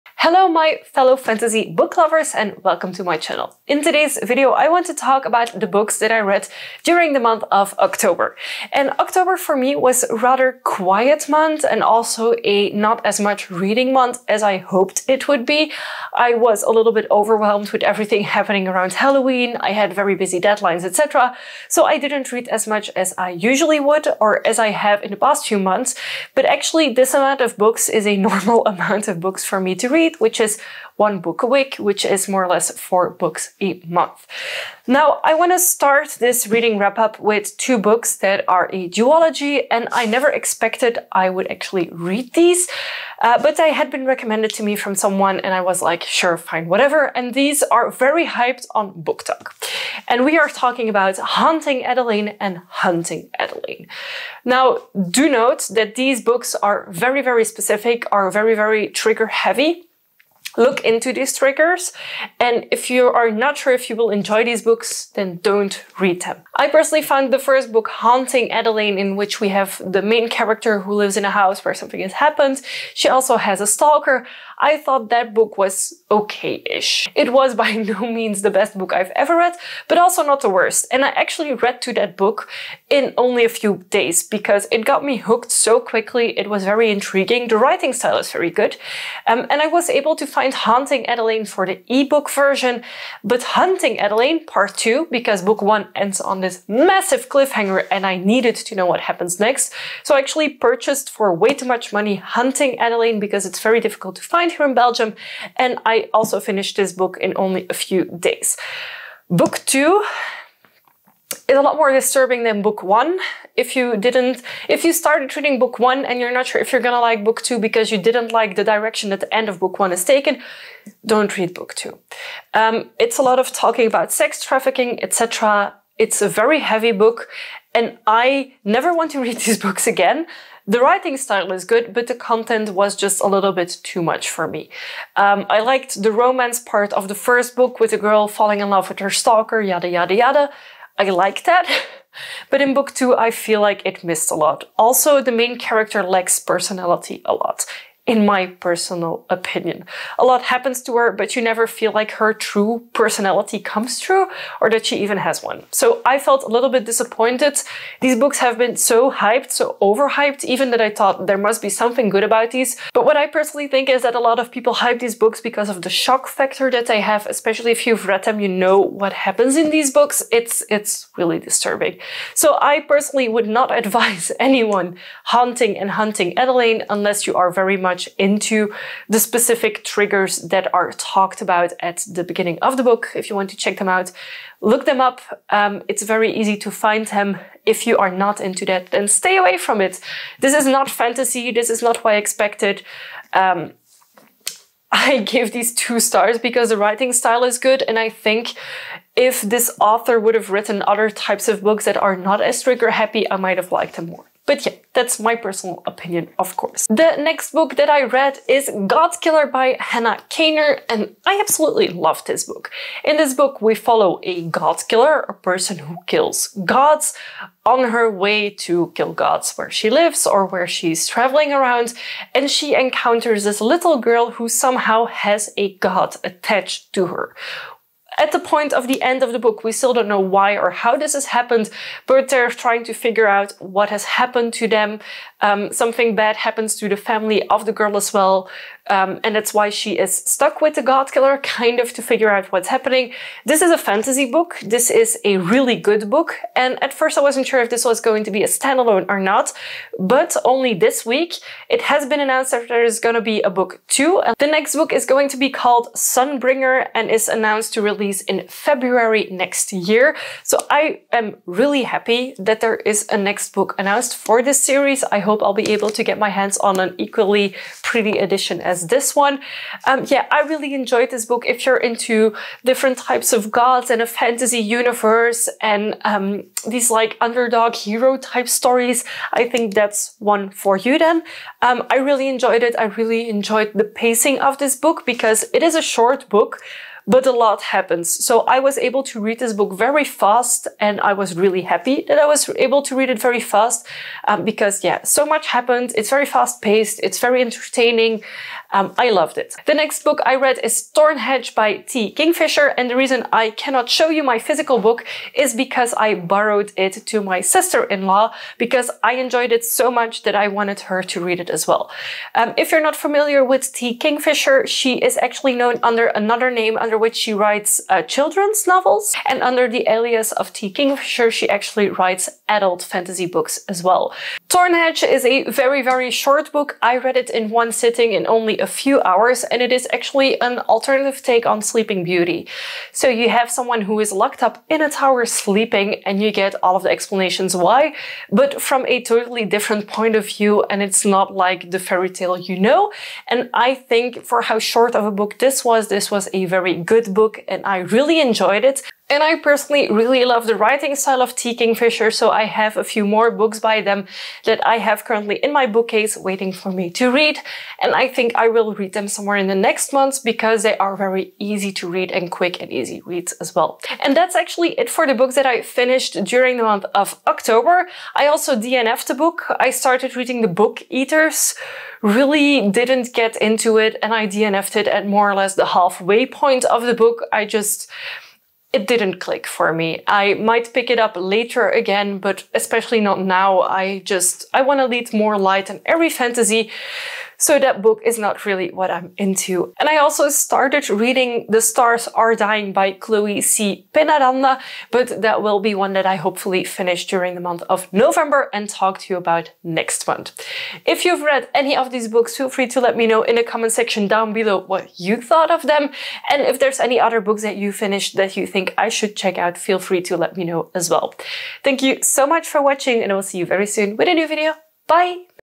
October fantasy reading wrap up
Let's talk about the book I read during October.

Books I finished:
Haunting Adeline & Hunting Adeline

DNF'd:
The book eaters - Sunyi Dean

Started but didn't finish (yet):
The stars are Dyting - Chloe C Penaranda (made it to 250/566 on oct 30th)

Most of the gorgeous fantasy jewelry I wear is (unless stated otherwise) from Ayame Designs.
(not applicable on already discounted items such as mystery bags)